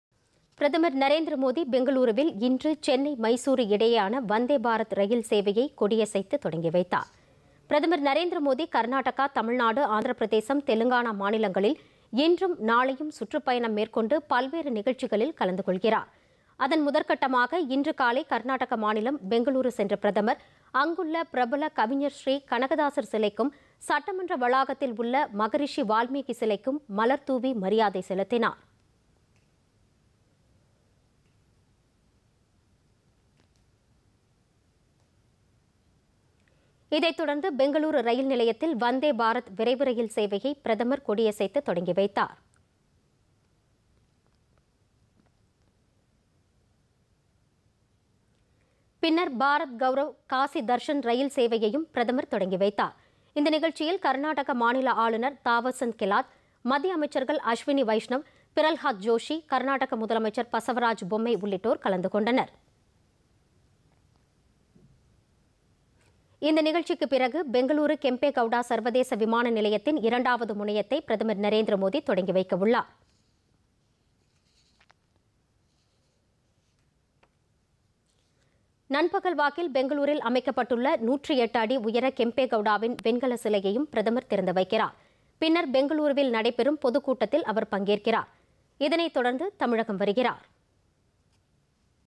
சென்னை-மைசூரு வந்தே பாரத் சேவை
சென்னை- மைசூரு இடையே வந்தே பாரத் விரைவு ரயில் சேவை - பெங்களுருவில் தொடங்கி வைத்தார் பிரதமர் நரேந்திர மோதி.